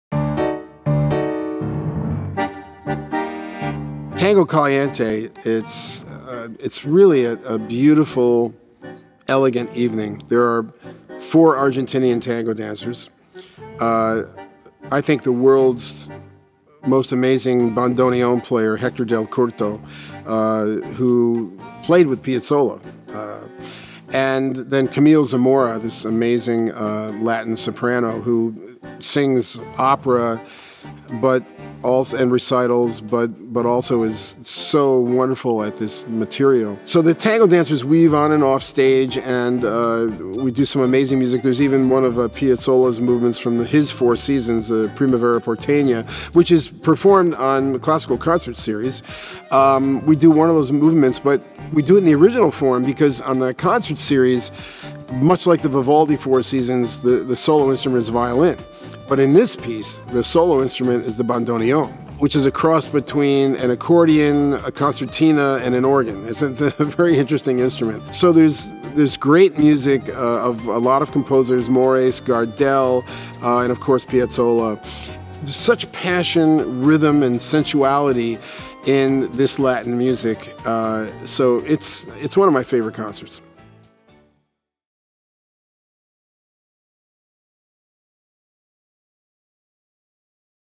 Astor Piazzolla & Tango Caliente
Principal Pops Conductor Jeff Tyzik tells us about the DSO's upcoming Astor Piazzolla & Tango Caliente performances. Join us May 4-6 for an evening of Argentine heat!